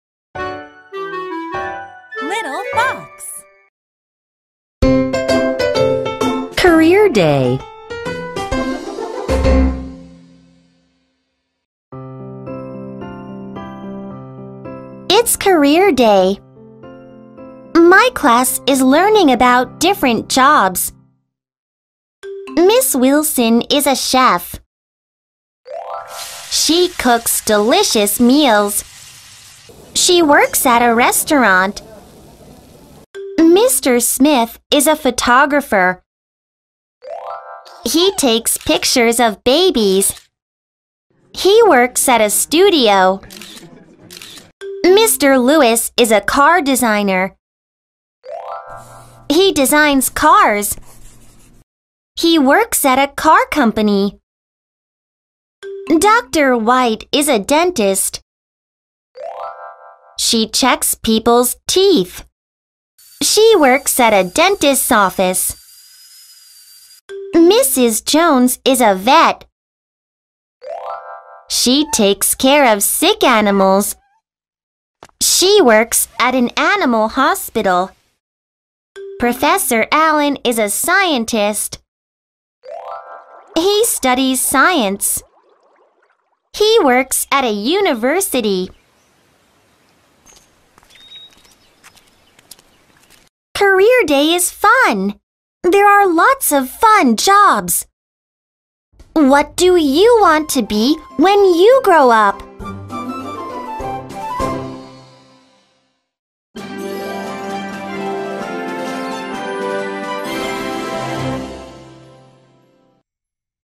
Career Day | Friendship | School | Little Fox | Bedtime Stories
Career Day

What do you want to be when you grow up? Many schools have Career Day. On Career Day, children can learn about different careers and locations.